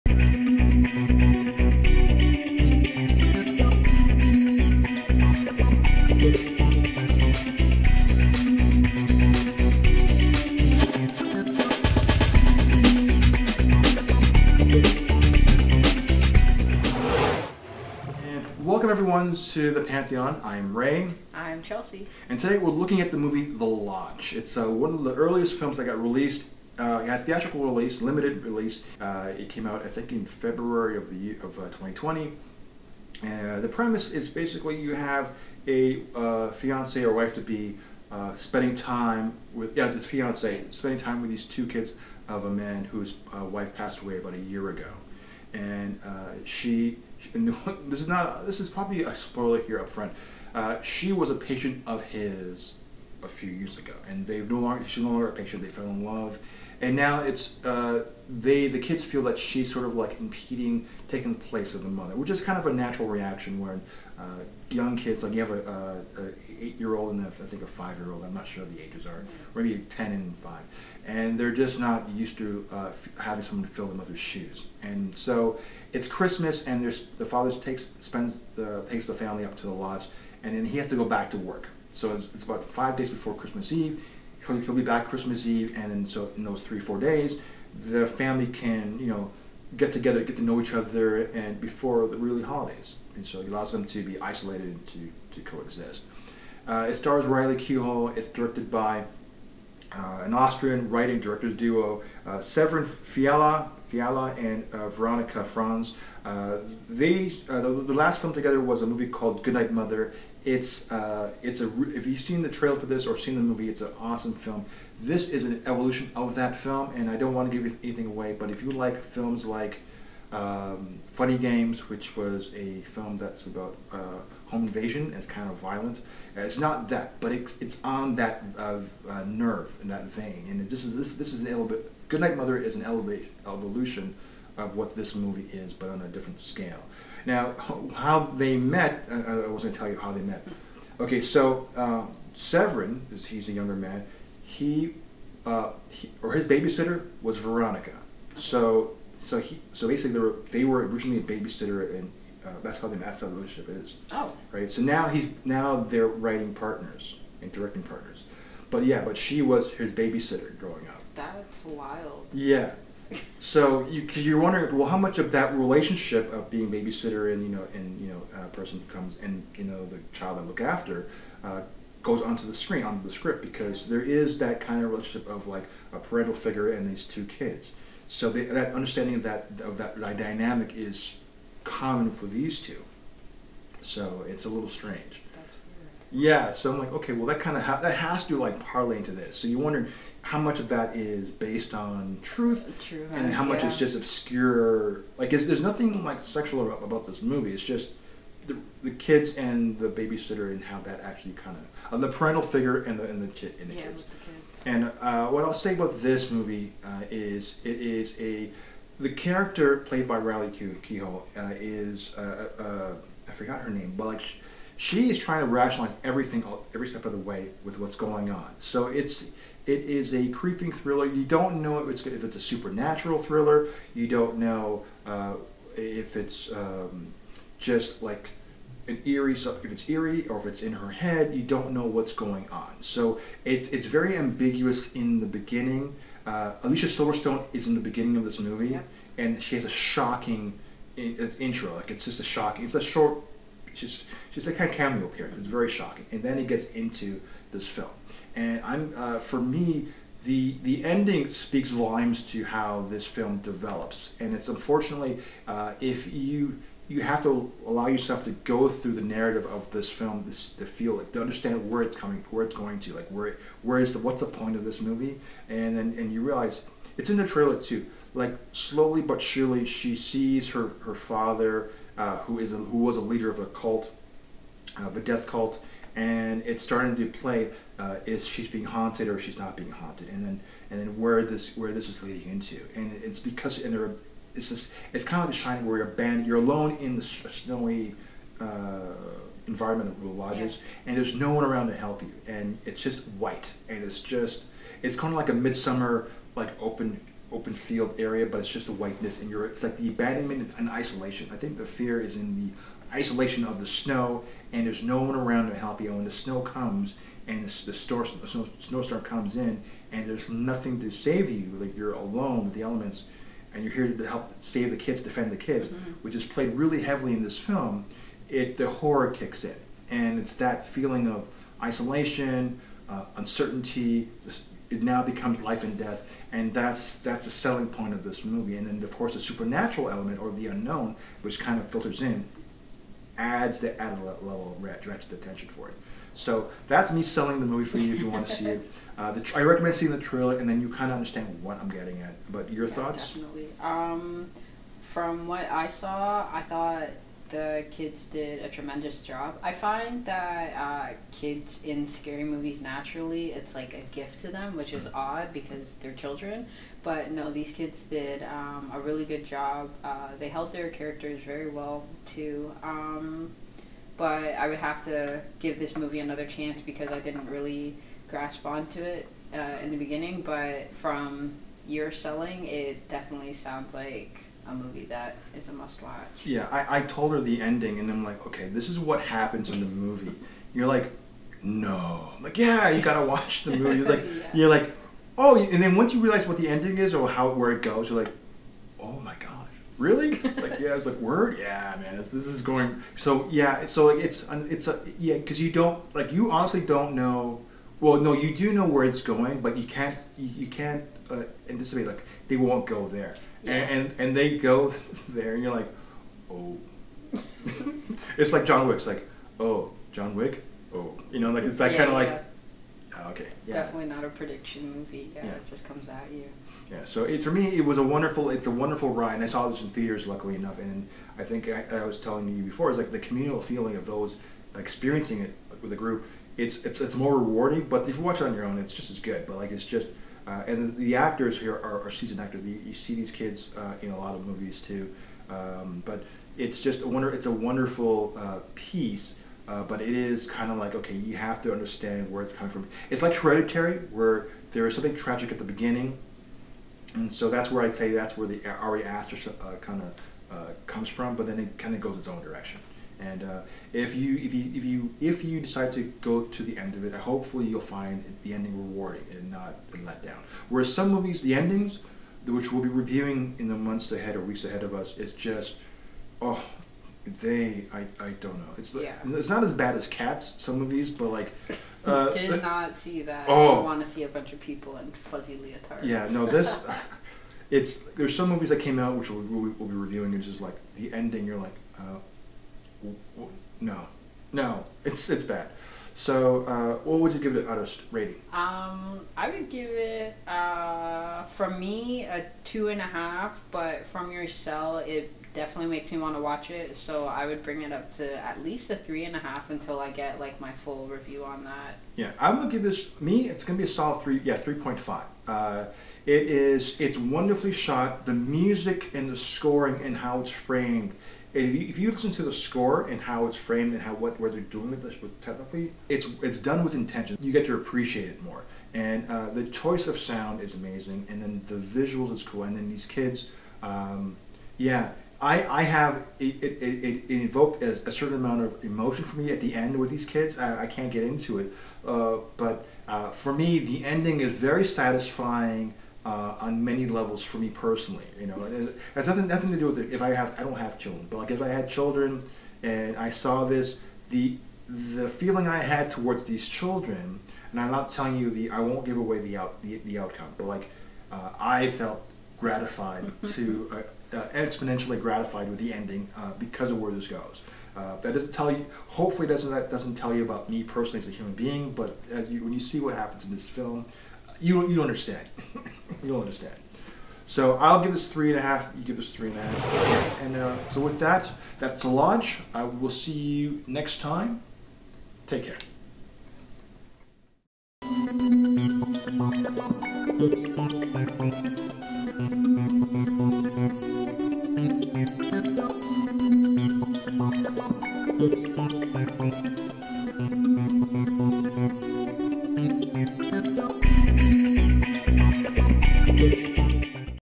The Lodge
October is our Days Till Halloween Review
With Special Co-Host Chelsey;  Today's Review The Lodge.